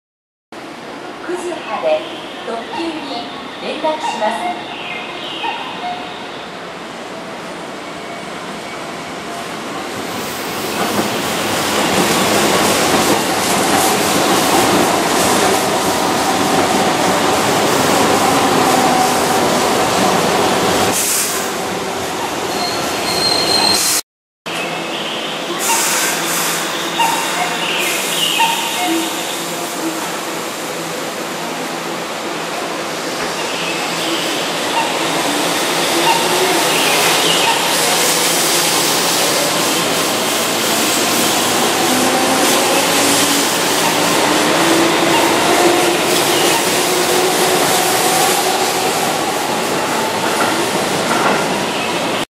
京阪電車＊＊＊９/２６ 朝の２６３２編成ちゃん
最近イケてる仕事を貰ってませんでした２６３２編成ちゃん、ようやく目立つ仕事が貰えましたのでＭ０８０３Ａの準急姿と折り返しのＦ０９０２Ｒの急行姿をつないでみました(笑）